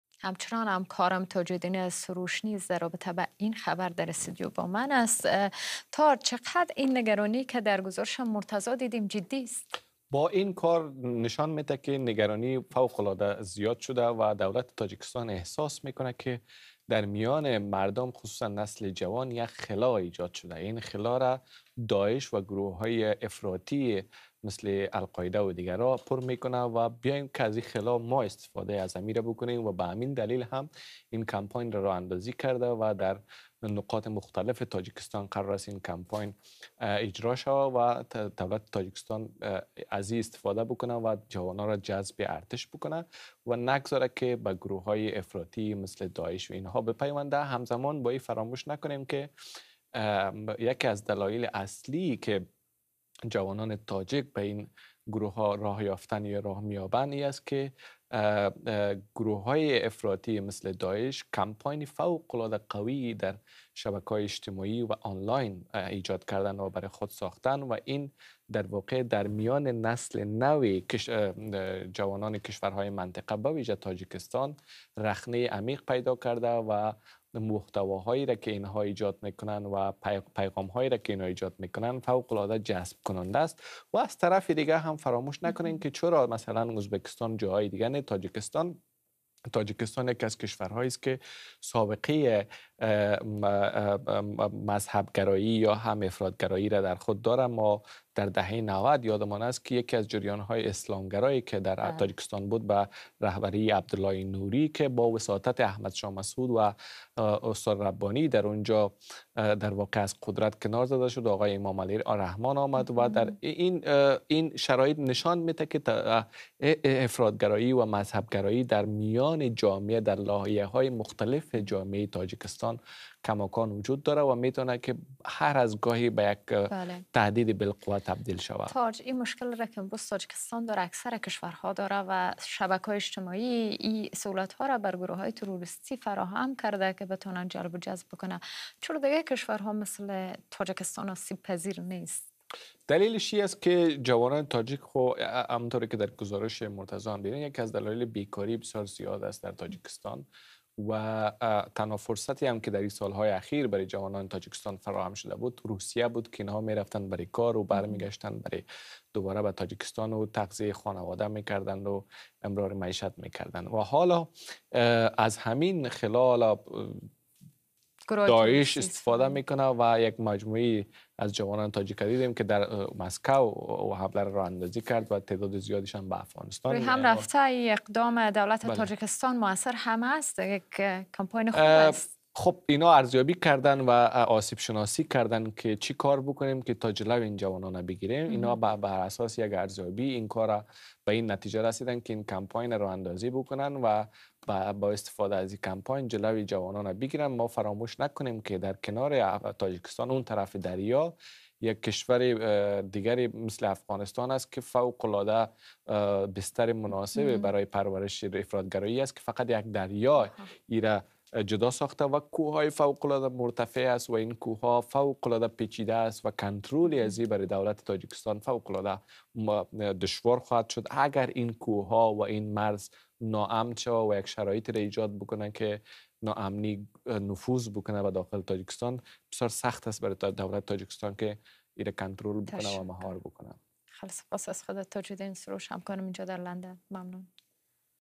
کارزار مبارزه با افراط‌گرایی و جلوگیری از پیوستن جوانان به داعش در تاجیکستان
توضیحات تاج‌الدین سروش، خبرنگار افغانستان اینترنشنال